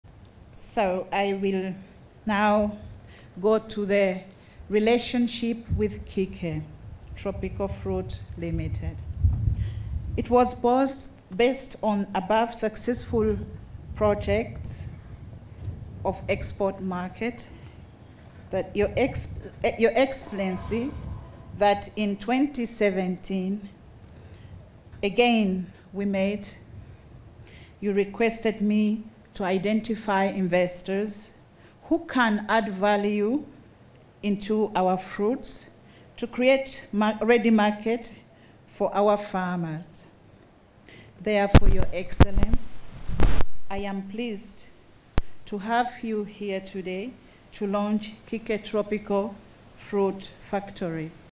It was based on above successful projects
Relationship with KIKE || It was based on above successful projects of export market || H.E Gen. Yoweri Kaguta Museveni commissioning Kike Fruit Processing Factory at Kakooge Nakasongola District on 02/11/2023.